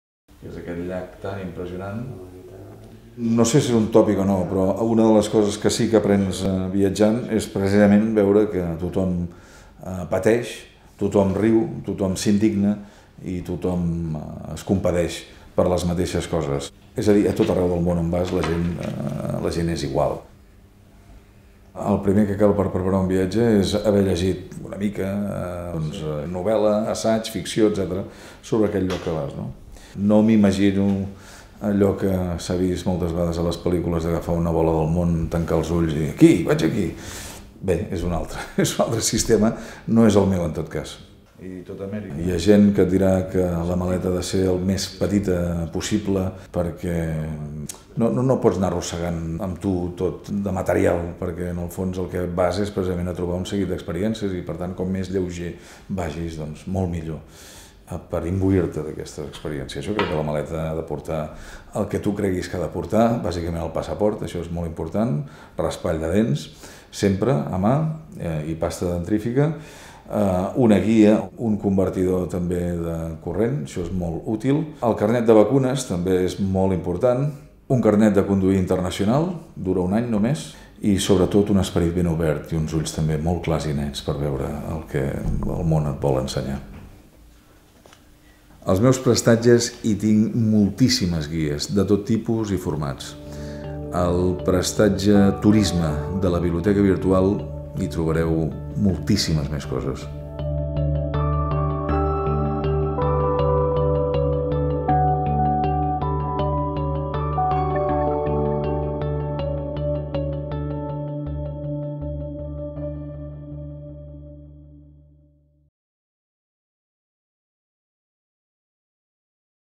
Prestatge virtual de Turisme: Miquel Calçada
L'aventura de viatjar de la mà d'en Miquel Calçada. Ens explica com prepara els seus viatges, que s'enduu a la maleta i ens aconsella tenir un  esperit obert i ulls clars i nets per veure el que el món ens pot ensenyar.